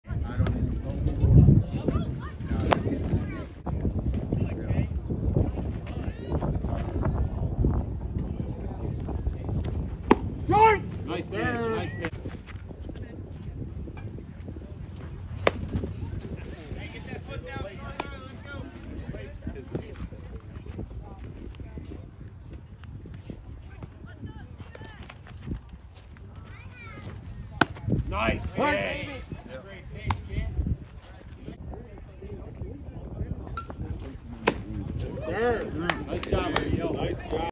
Akhil Pilla-12u Baseball-12u PG showcase In Palmyra,PA last weekend- Looking forward to13u Fall-ball
Highlights from Akhil's pitching outing at 12u PG showcase from past weekend (8/12 to 8/14). He had a great outing. Sorry for the bad camera angle and watch out the next video of his batting highlights